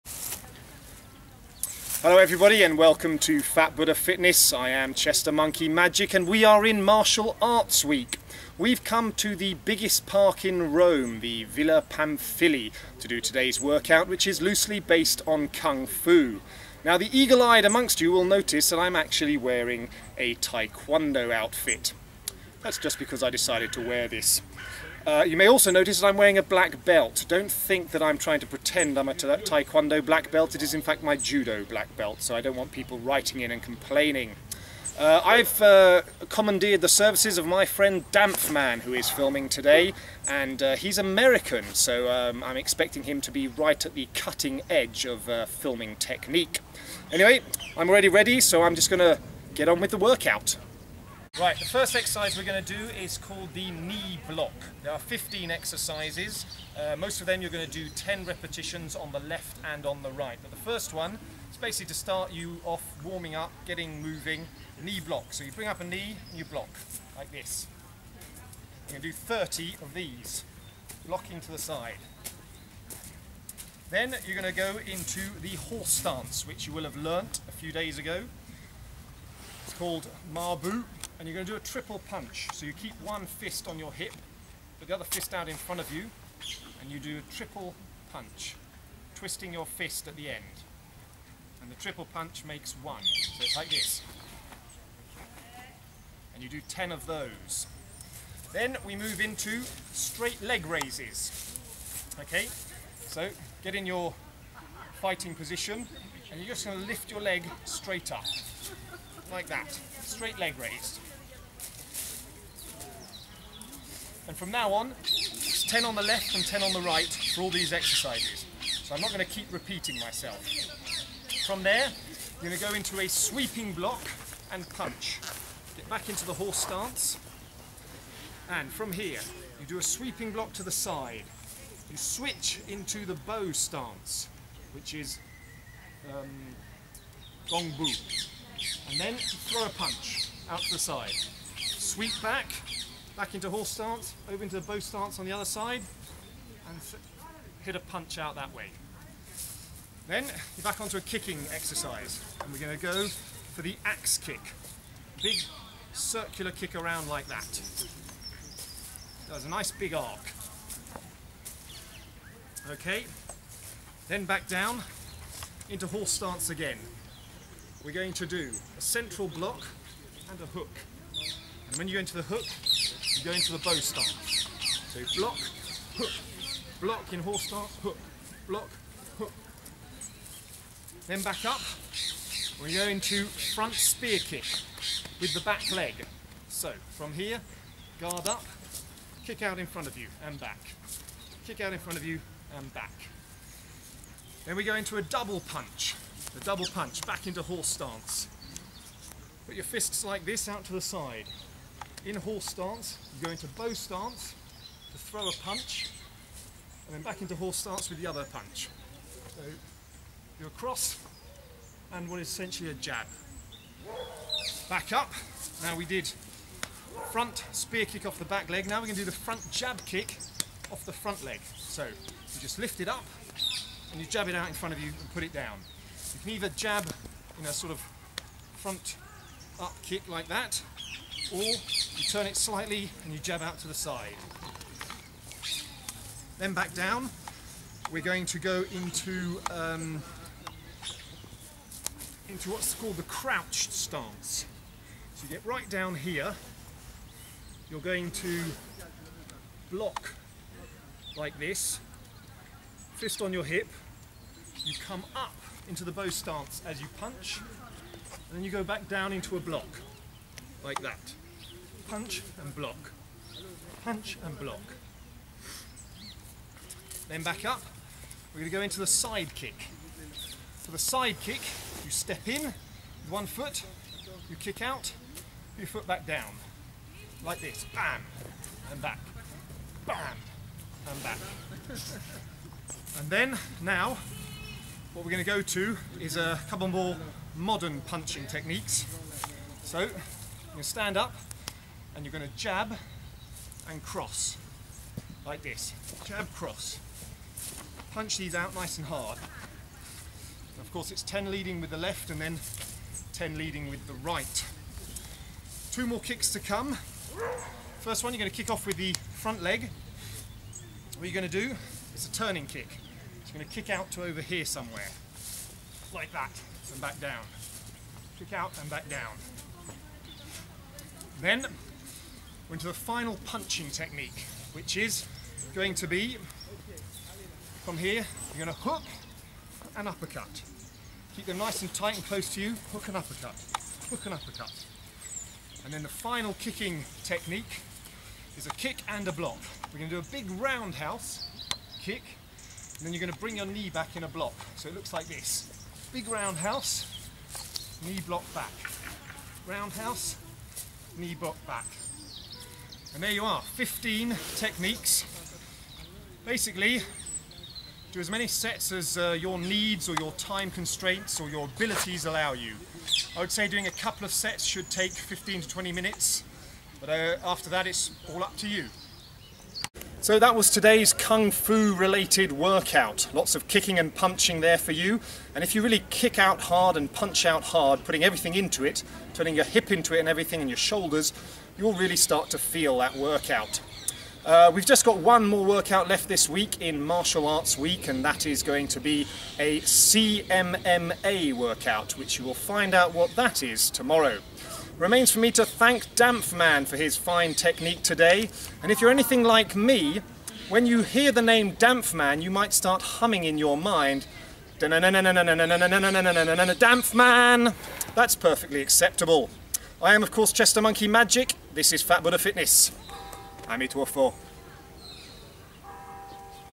Kung Fu Workout
Yesterday we gave you a judo workout which was a tricky one to put together as judo is a sport that really needs to be practised with a partner. Today's Kung-Fu workout is much easier to do on your own because it involves kicking and punching and these are exercises that can easily be done in thin air. We've called this a Kung-Fu Workout because there are a few traditional Kung-Fu techniques in it but most of it could equally be used in a kickboxing, karate or taekwondo workout amongst others. As ever with kicking and punching exercises don't just go through the motions, actually strike with power or you won't be getting the full benefit of the workout. This is essentially a mix of punching and kicking although certain exercises are done in the stances you did on Tuesday so this will really work your legs. If you want to learn more about kung fu then visit this for workout videos on the shaolin arts or you can try this book if you prefer just learning about the Chinese Art.

Do two or three sets of the following:

Knee-blocks x 30
Triple punch x 10
Straight leg raises x 20 (10 on each leg)
Sweeping block and reverse punch x 20 (10 in each direction)
Axe kick x 20
Central double block and hook x 20
Front spear kick x 20
Side jab and reverse punch x 20
Front jab kick x 20
Crouching block and reverse punch x 20
Side kick x 20
Jab and cross x 20 (10 with each arm leading)
Turning kick x 20
Hook and uppercut x 20
Roundhouse and knee block on return x 20

This will last a bit longer than some of our other workouts so if you have limited time then just do one set but if you can do more, all the better.

Amituofo

Chestermonkeymagic